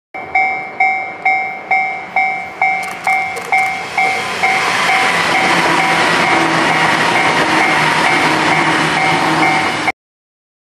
【黒船電車】特急リゾート踊り子号 東海道線 平塚～茅ヶ崎間（須賀道踏切）
伊豆急行リゾート21（黒船電車）を使用したリゾート踊り子号　東京行き
2009年頃
昔撮り貯めた鉄道動画シリーズその46